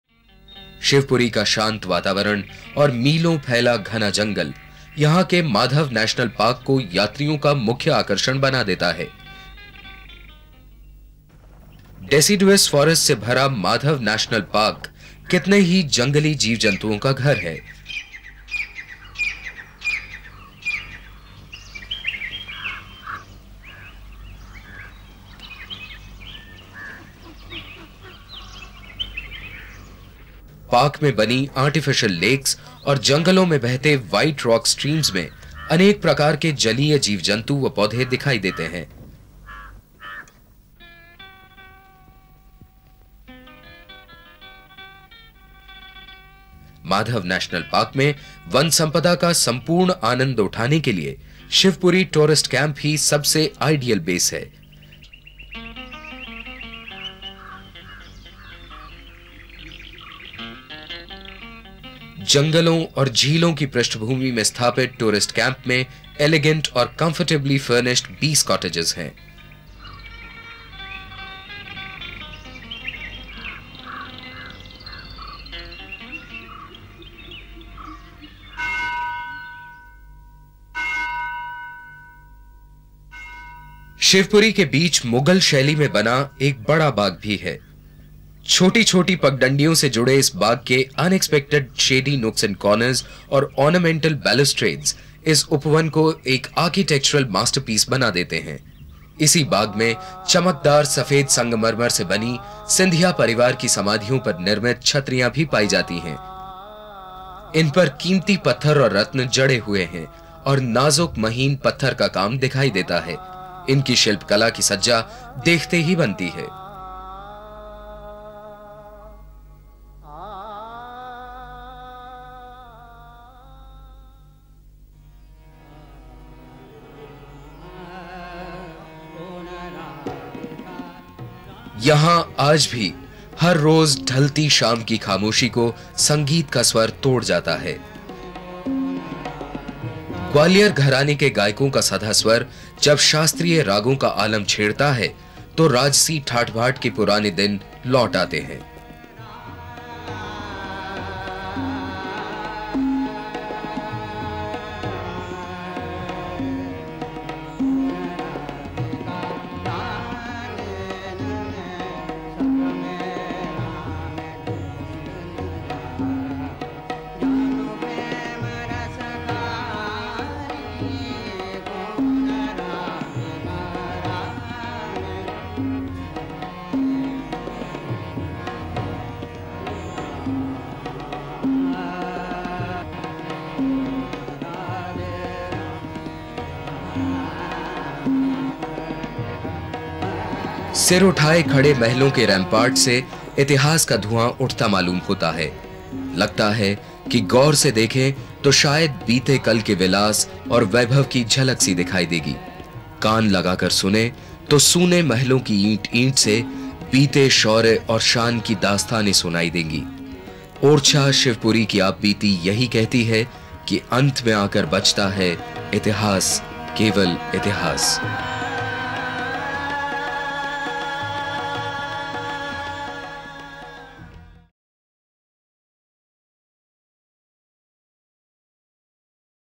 Madhav National Park and the heritage of the Scindia dynasty
Herds of Chital or spotted deer graze while tiger cubs play amongst themselves.

The calmness of the Shivpuri district and the forest cover around the Madhav National Park is what attracts the tourist to this place in Madya Pradesh.

The deciduous forest in Madhav National Park is the home to quite a few animals. The artificial lakes in the park and white rock streams in the forest give life to hundreds of birds and aquatic animals.

To make the most of Madhav National Park, the 'Shivpuri Tourist Camp' is the most ideal place to stay.
Amidst the National Park lies 'Bada Bagh', which is constructed using marble and has amazing architectural nooks and corners and ornamental balustrades. Here is where one can find the sepulcher of the Scindia dynasty.